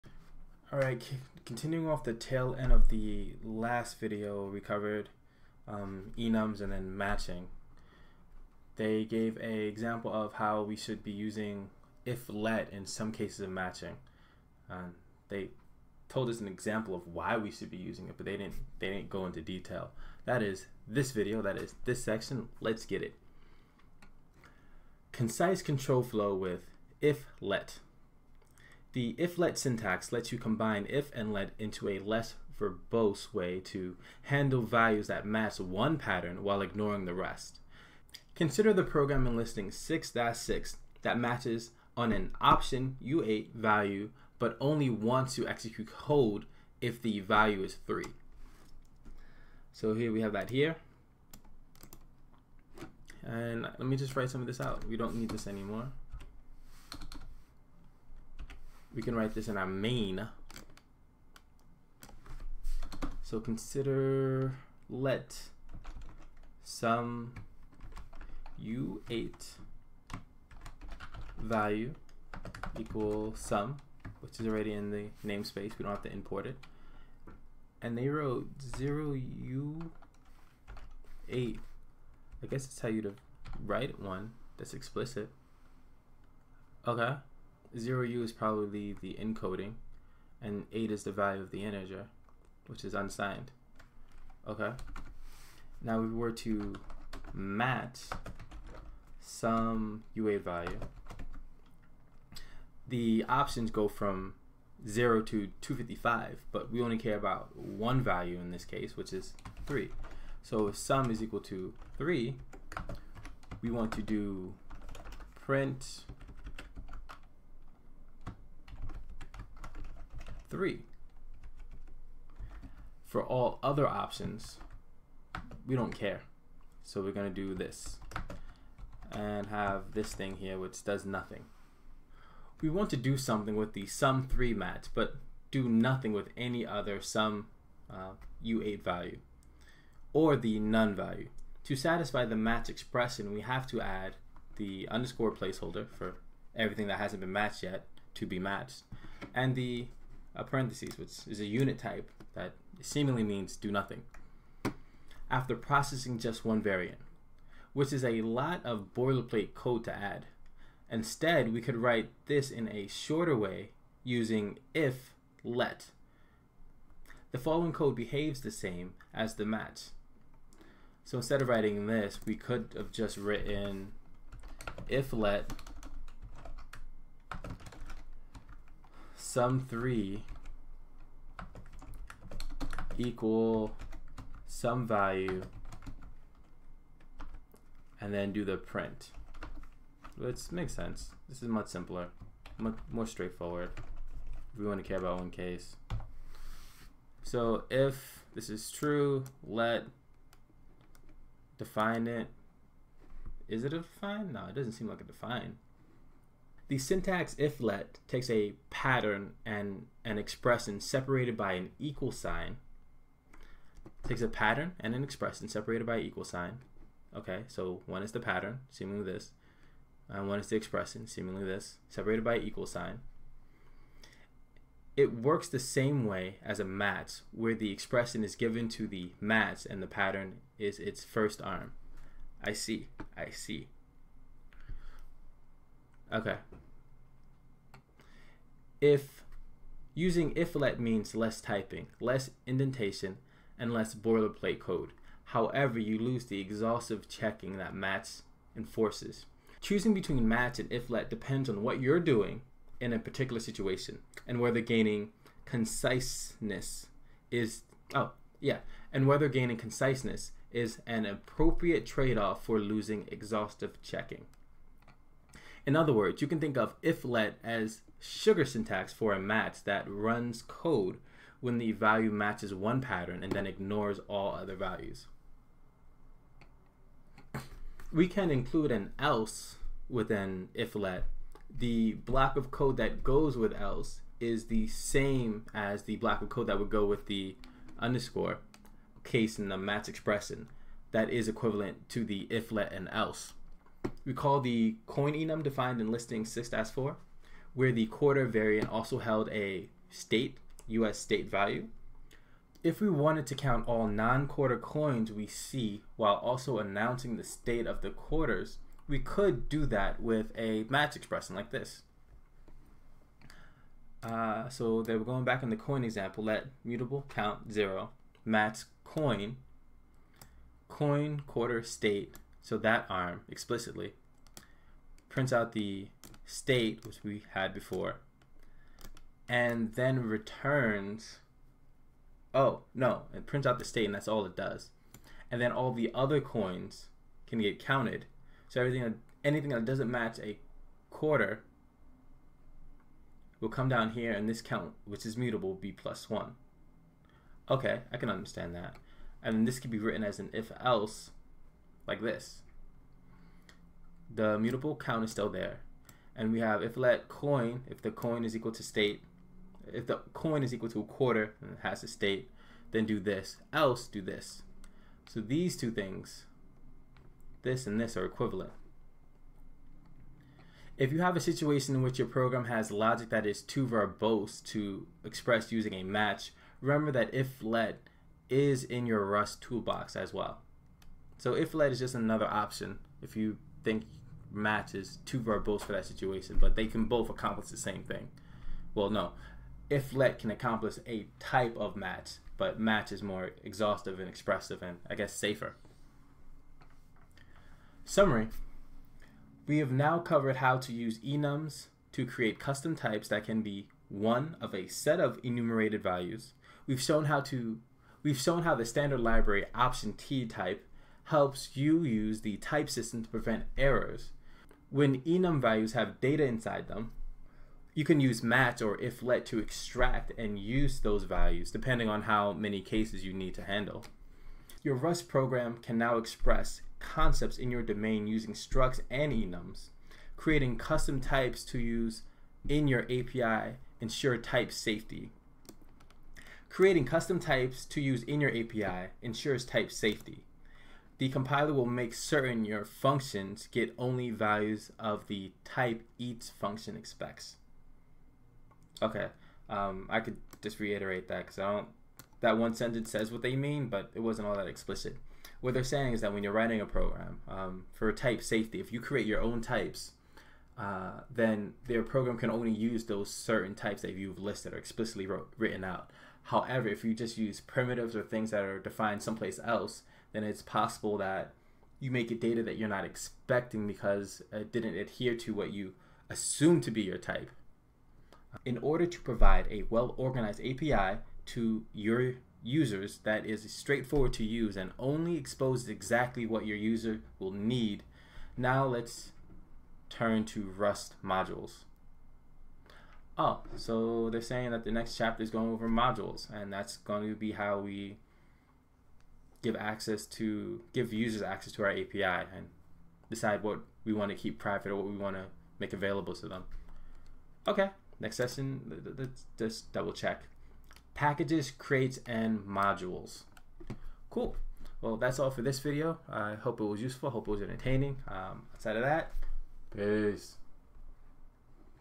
Rust Enums Part 3: if let (Syntax)
Continuing "The Rust Programming Book" Chapter 6 on using the if let syntax.